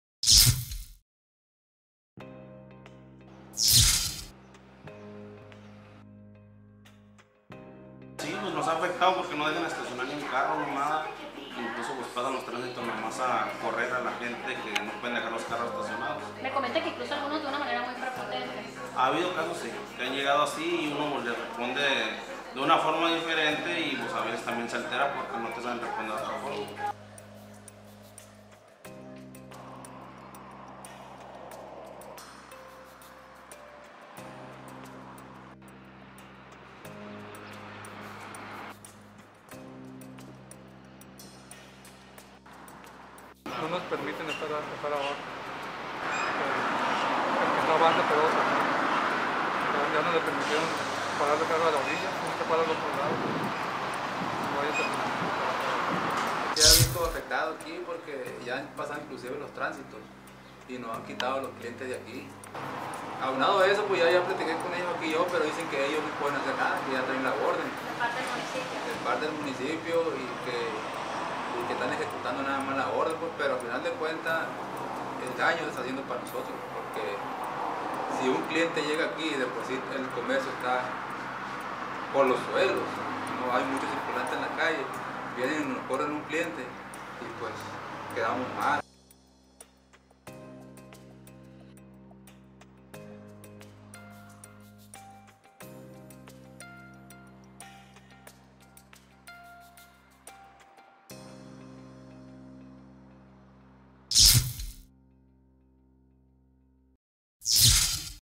Comerciantes de la Leyva cerca de desaparecer; no hay estacionamiento
En el 2011, se ordenó que la calle Gabriel Leyva Solano del tramo del bulevar Centenario al Rosendo G. Castro fuera de doble sentido y se limitaron las áreas de estacionamiento. Desde entonces los comerciantes del lugar luchan por sobrevivir; otros más no lo lograron.